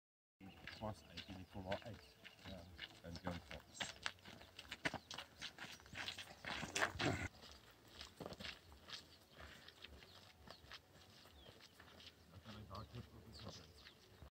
15 September 2018 Nieu Bethesda to Graaff Reinett (StreetBiz Shares: The Long Walk Project)
15 September 2018, The Long Walk Project ... on the road from Nieu Bethesda to Graaff-Reinett, 15 September. A group of local residents joined Nico when he started the walk to Graaff-Reinet.